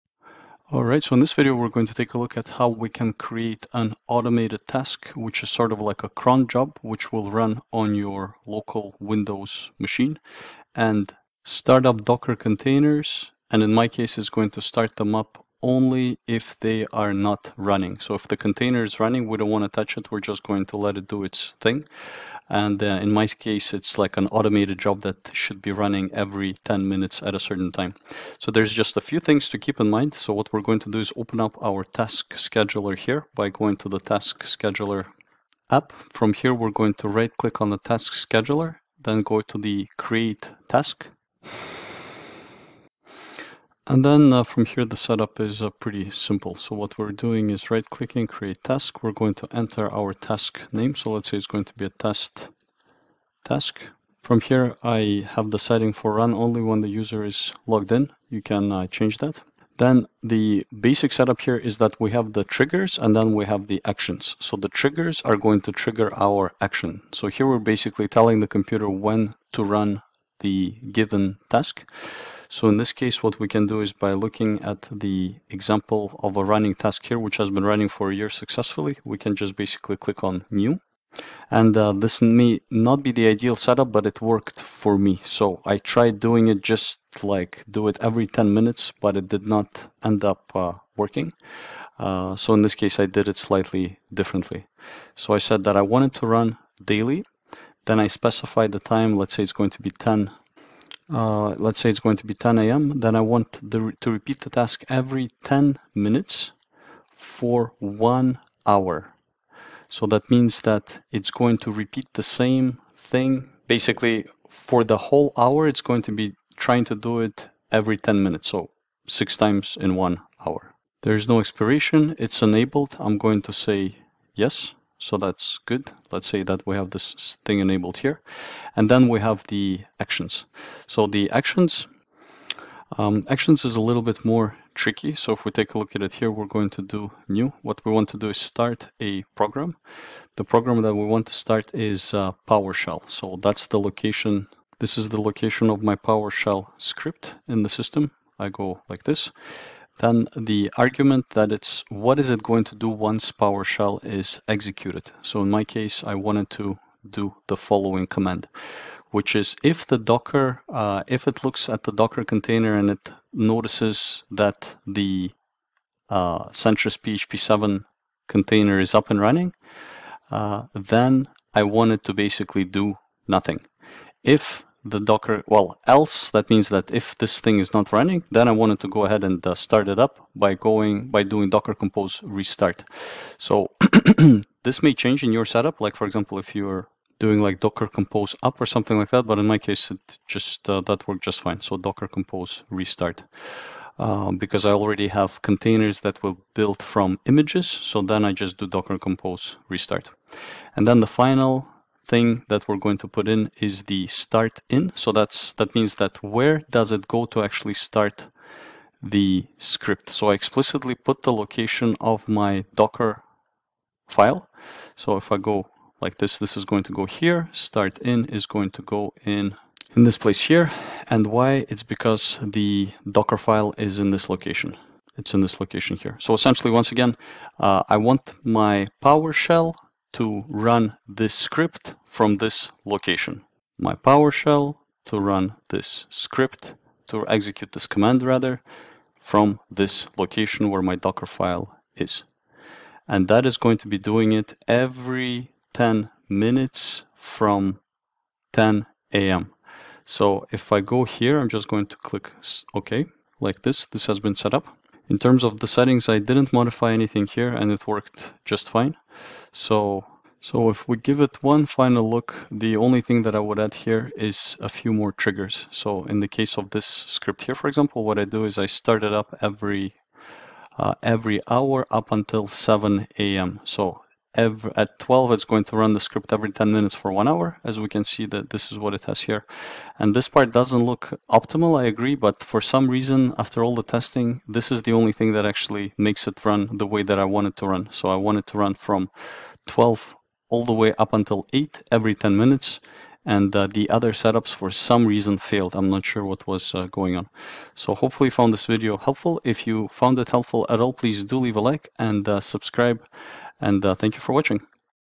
Automatically start docker on a schedule via cron (task scheduler)
Commands in the video (adjust to your environment):

%SystemRoot%\system32\WindowsPowerShell\v1.0\powershell.exe

if ( docker ps | Select-String -quiet CONTAINER_NAME ) {} else { docker-compose restart }

C:\PATH_DO_YOUR_DOCKERFILE